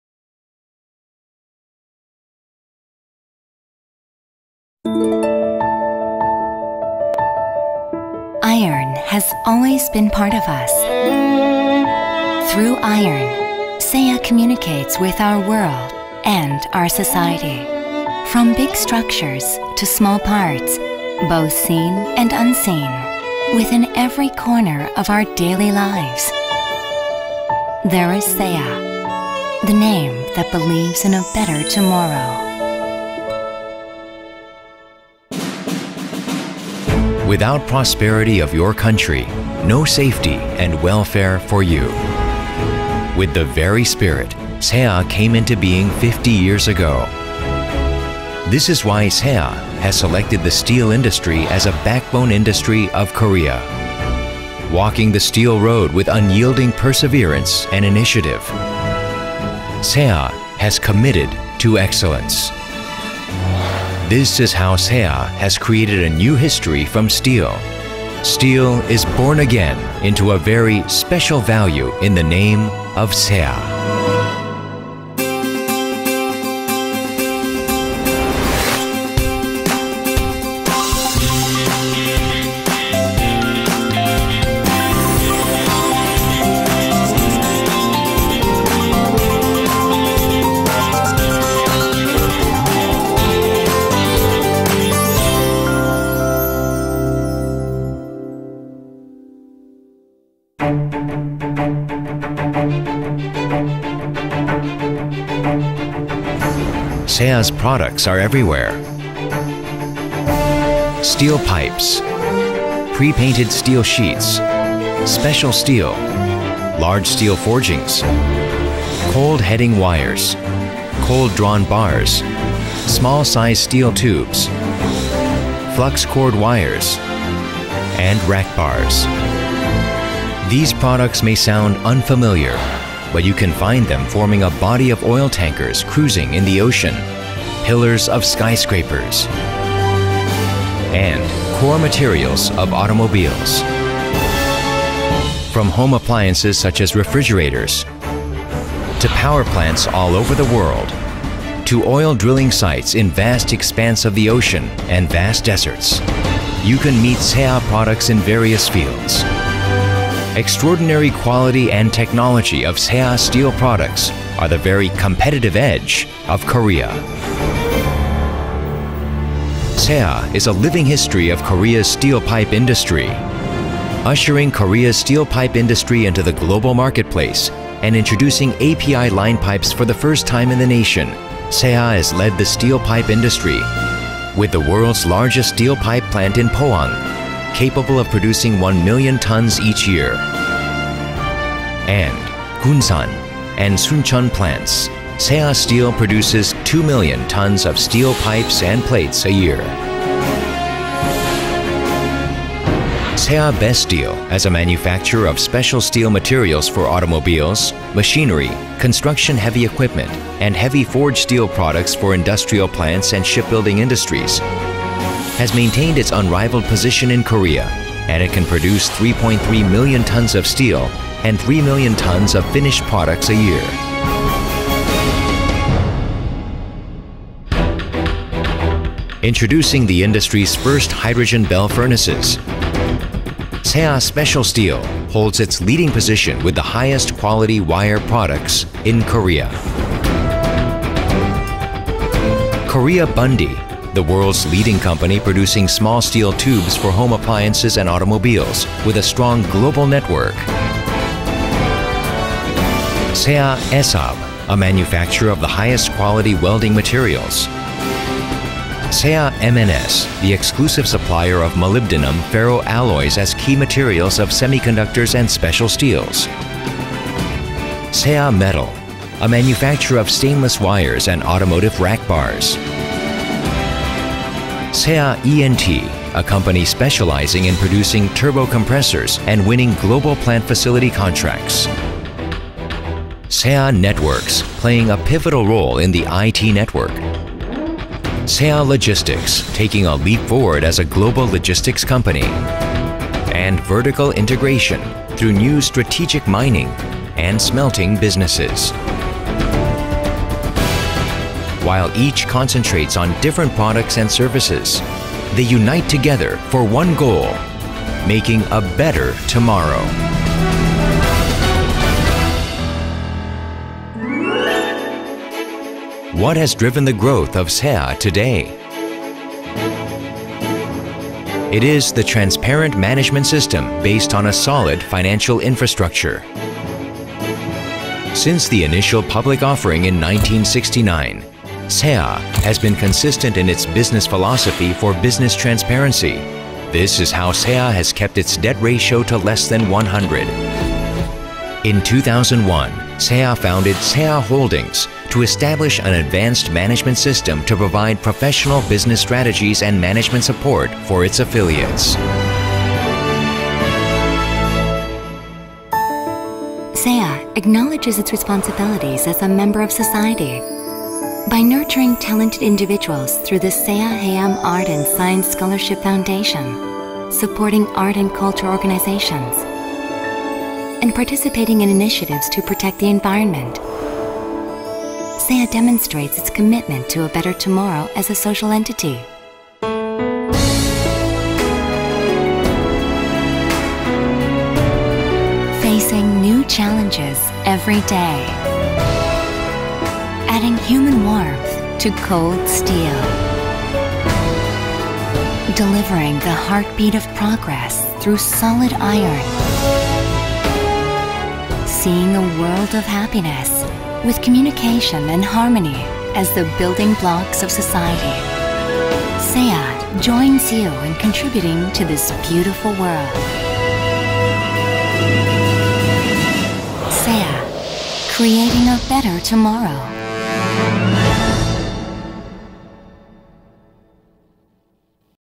SeAH Grup Şirketleri Tanıtım
Türkiye temsilcisi olduğumuz Güney Kore merkezli SeAH holding bünyesinde bulunan SeAH Engineering firması hava yataklı turbo blower ve turbo kompresör üretmektedir.